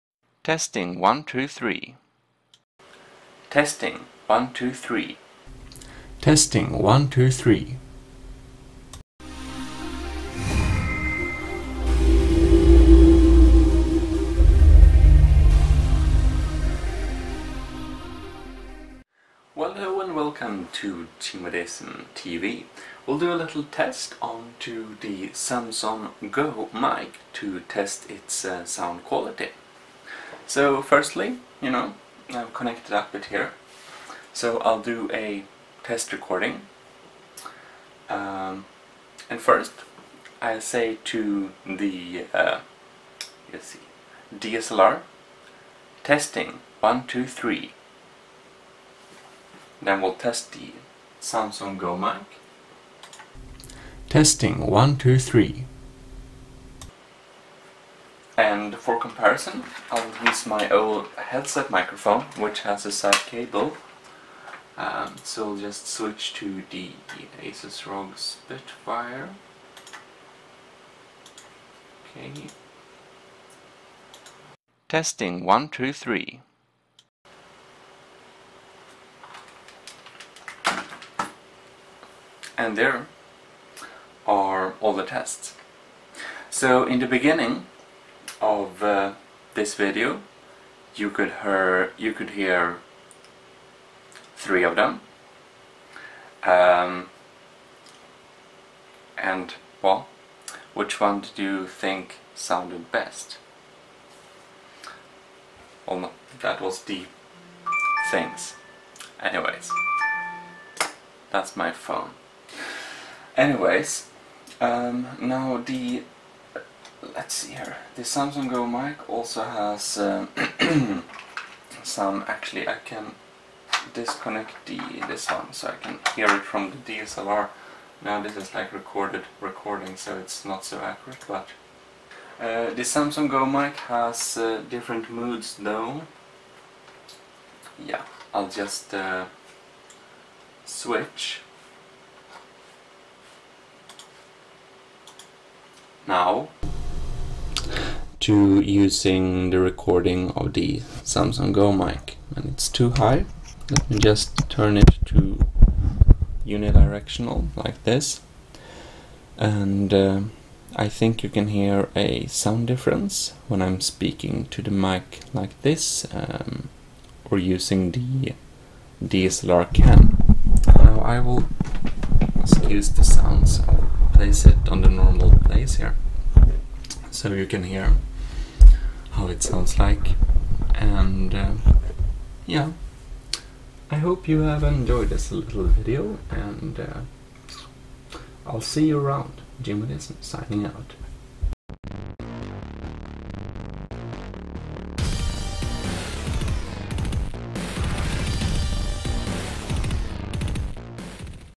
Samson Go Mic Sound Testing! - Portable USB Microphone for Recording - Review
The Sound Testing of the Samson Go Mic, some say the best USB mic available for the money, and in league or better than the Blue Yeti.
Hope it will serve me well! :D
The Samson GO mic in BLACK:
Even smaller model (I have not tested this one yet):

-Enjoy! ;D